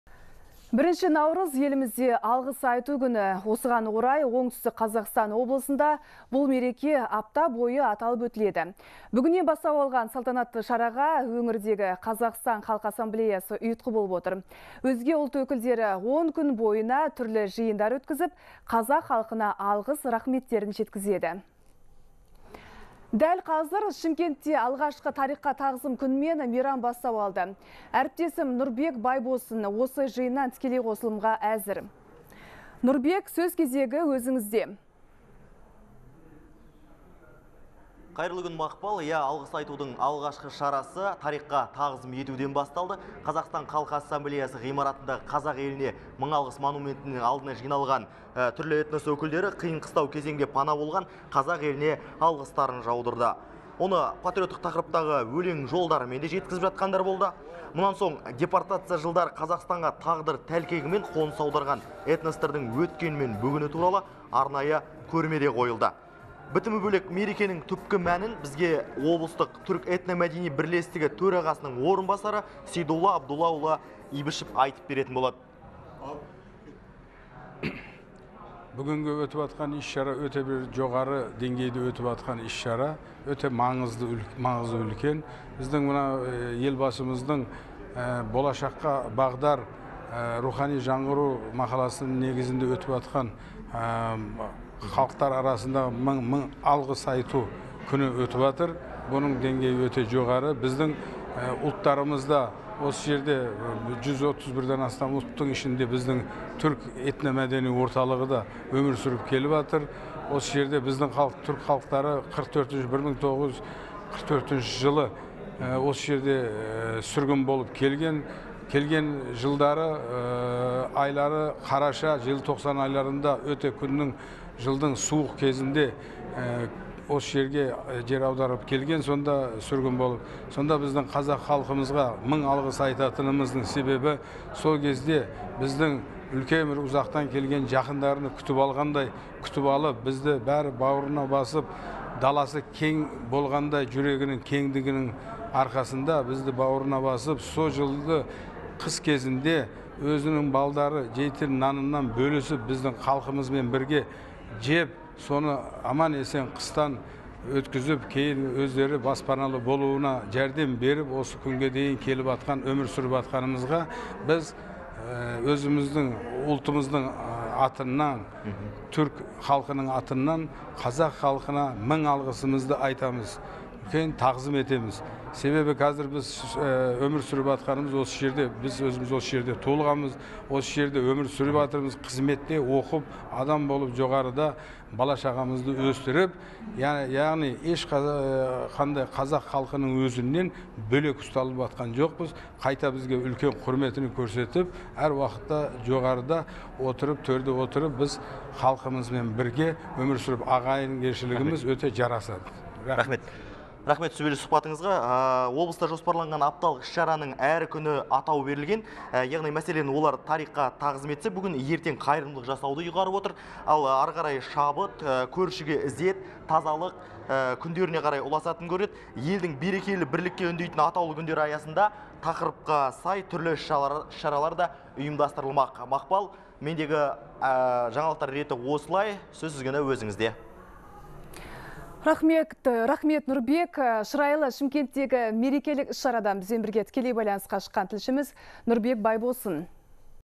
ОҚО-да алғыс айтудың алғашқы шарасы «Тарихқа тағзым» етумен басталды
Бірінші наурыз – елімізде Алғыс айту күні.

Осыған орай Оңтүстік Қазақстан облысында бұл мереке апта бойы аталып өтіледі. Бүгіннен бастау алған салтанатты шараға өңірдегі Қазақстан халқы Ассамблеясы ұйытқы болып отыр. Өзге ұлт өкілдері 10 күн бойына түрлі жиындар өткізіп, қазақ халқына алғыстарын жеткізеді.

Сонымен қатар Шымкентте алғашқы «Тарихқа тағзым» мейрамы бастау алды.

«Қазақстан халқы Ассамблеясы» ғимаратындағы «Қазақ еліне мың алғыс!» монументінің алдында түрлі этнос өкілдері қиын-қыстау кезеңде пана болған қазақ еліне алғыстарын жаудырды. Оны патриоттық тақырыптағы өлең жолдарымен жеткізді. Депортация жылдары Қазақстанға тағдыр тәлкегімен қоныс аударған этностардың өткені мен бүгіні туралы арнайы көрмеде қойылды.